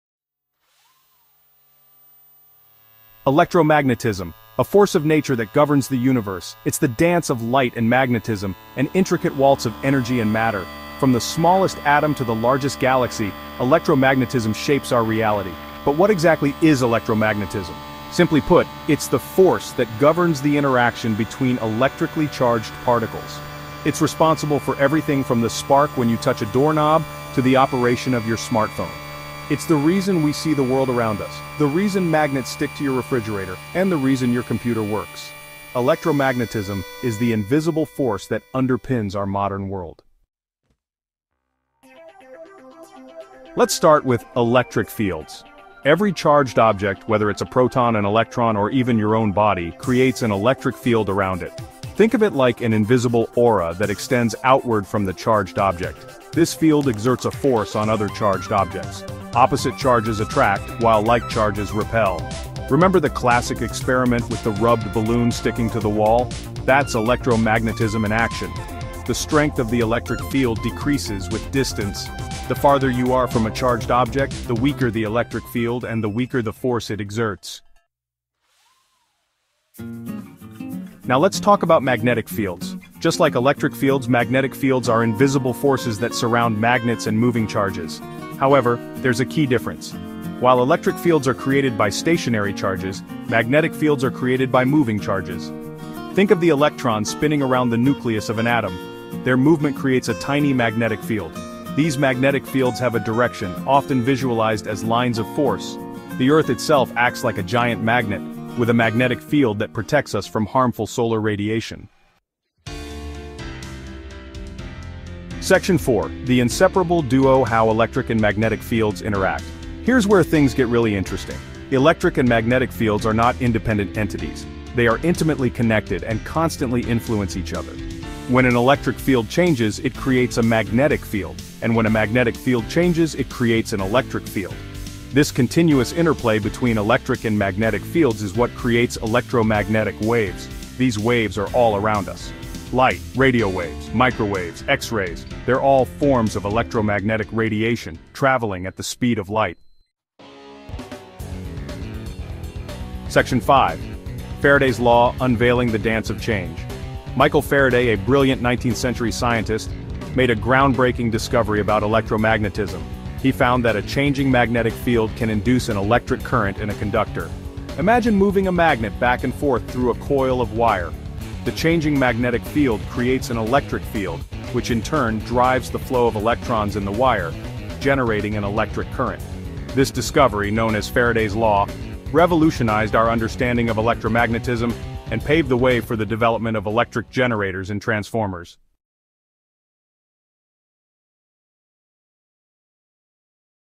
Unlocking the Secrets of Electromagnetism!
Dive into the fascinating world of electromagnetism as we unlock its secrets!
In this video, we’ll explore the fundamental physical laws that govern electromagnetic forces, from Coulomb's Law to Faraday's Law of Induction.
Discover how electric and magnetic fields interact, and how these principles power everything from household appliances to advanced technology.
Whether you're a science enthusiast or a curious learner, this journey through the principles of electromagnetism will enlighten you about the forces that shape our universe.

OUTLINE:

00:00:00 The Dance of Light and Magnetism
00:00:40 The Force of Attraction and Repulsion
00:01:17 The Enigmatic Aura
00:01:52 How Electric and Magnetic Fields Interact
00:02:29 Unveiling the Dance of Change